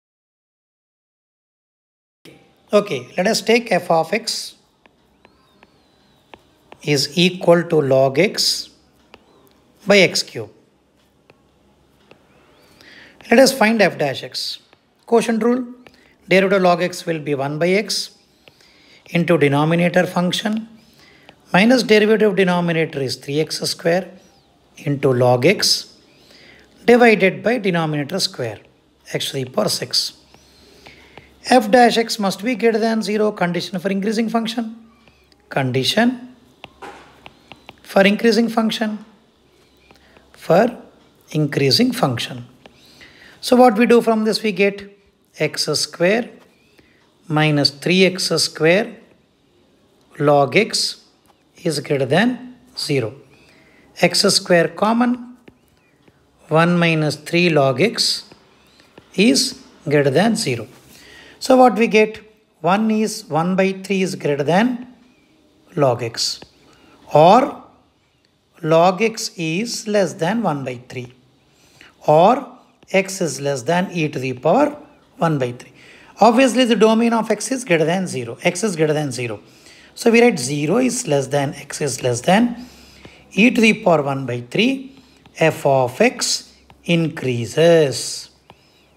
PU 2 | Stage 1 | Maths | AOD | Lecture 3 | Slide 1 | EX 1
Welcome to Concept Videos for PU 2! This slide will explain Example 1 of AOD. We are sure you will have an effective learning experience.